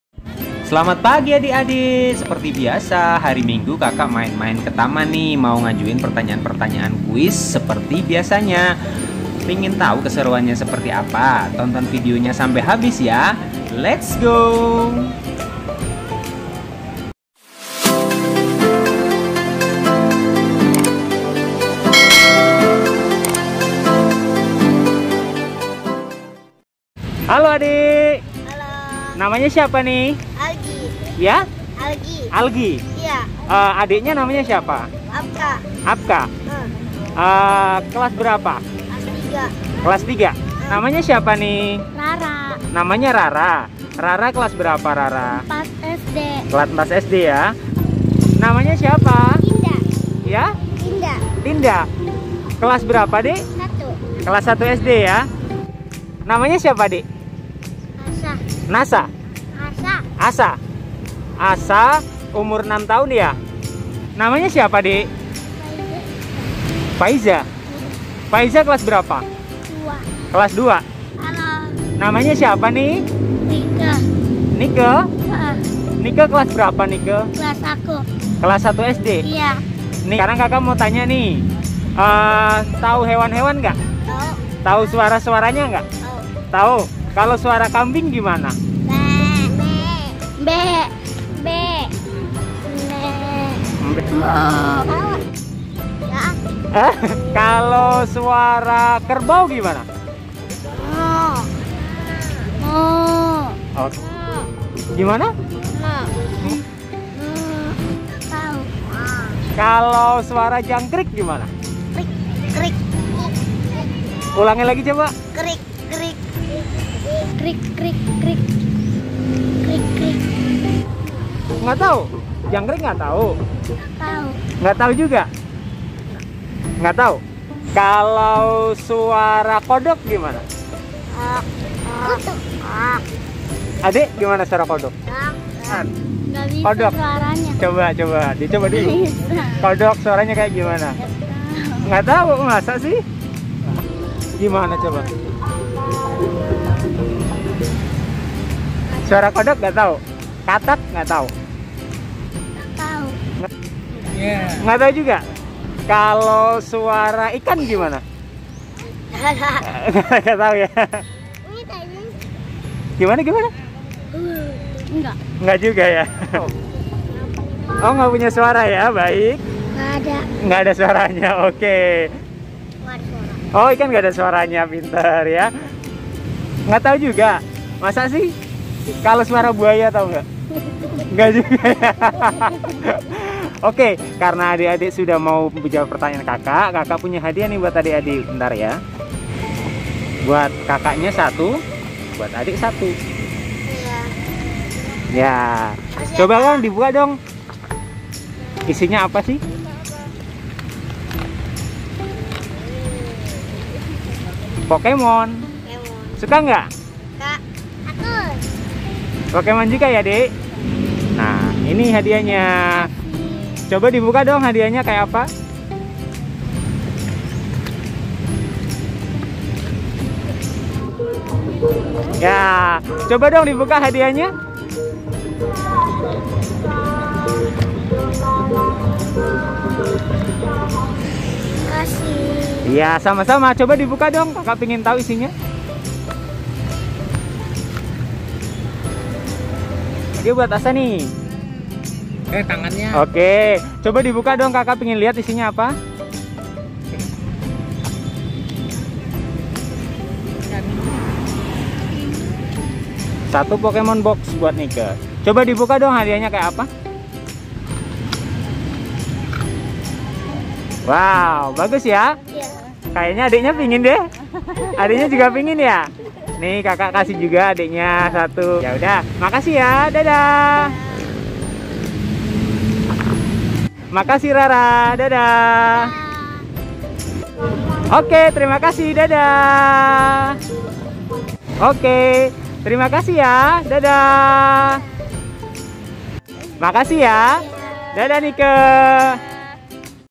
KATO quiz - kita cari tau suara2 hewan di sekitar kita
Random kuis untuk anak - anak di taman bermain